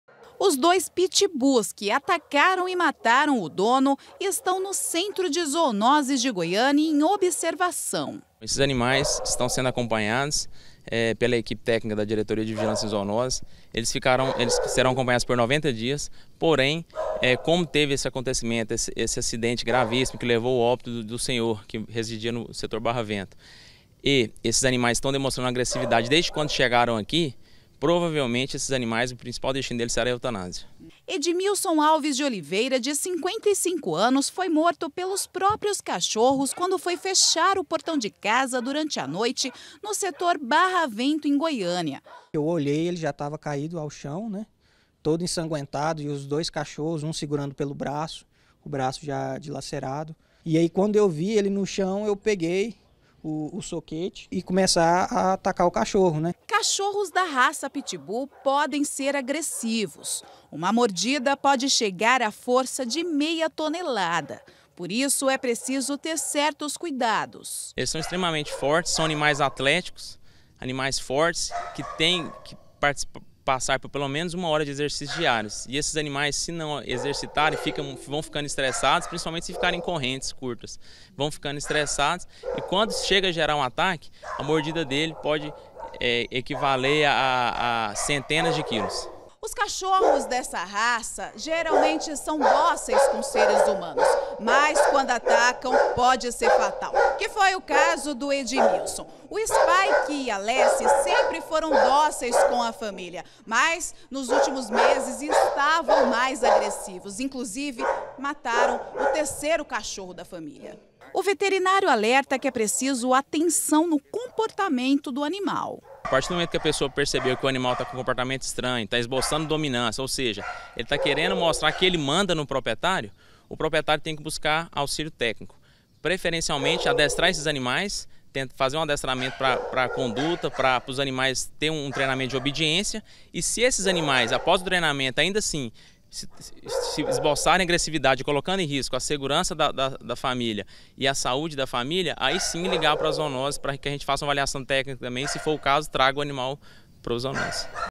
CÃES MATAM O DONO: ANIMAIS SÃO LEVADOS PARA O CENTRO DE ZOONOSES
E os dois cachorros que atacaram o senhor Edmilson foram levados para o centro de zoonoses de Goiânia. O veterinário que está acompanhando os animais disse que eles ainda estão muito agressivos.